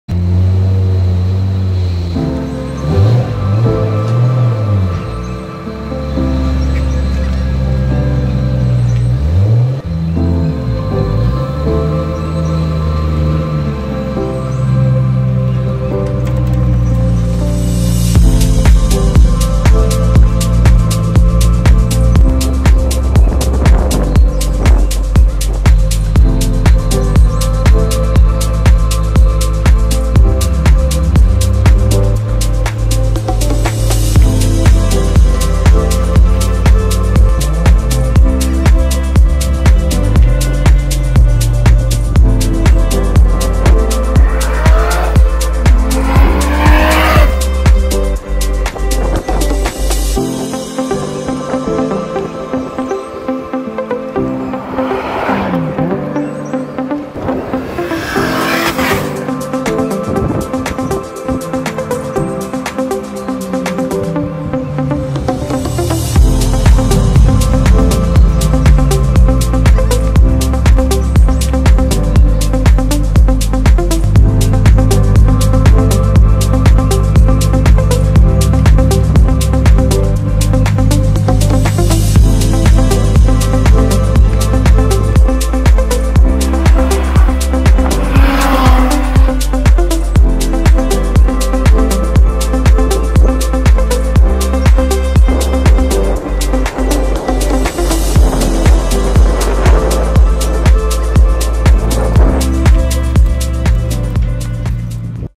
SynGarage Getting Ready for Drivermotorsports Japan Auto Legends!
Be sure to check out our booth at the event and watch mike rip his R32 GTR around VIR!